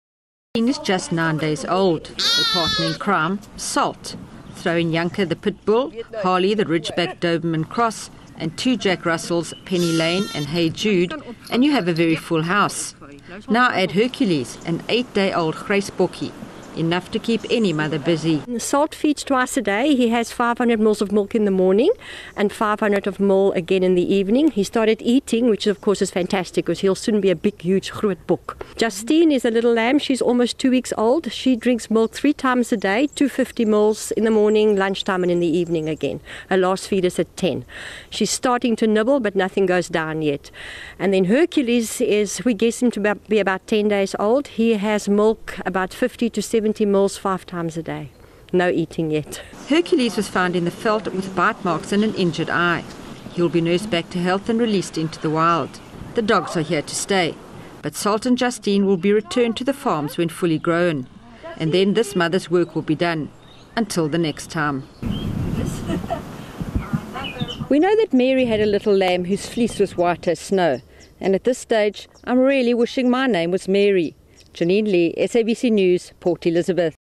Children of a different species being hand-reared in PE
Raising children can be difficult.
Now imagine taking care of children of a different species.
A lamb, a duckling and a baby goat - or a kid - were abandoned by their mothers and are now being hand-reared in Port Elizabeth.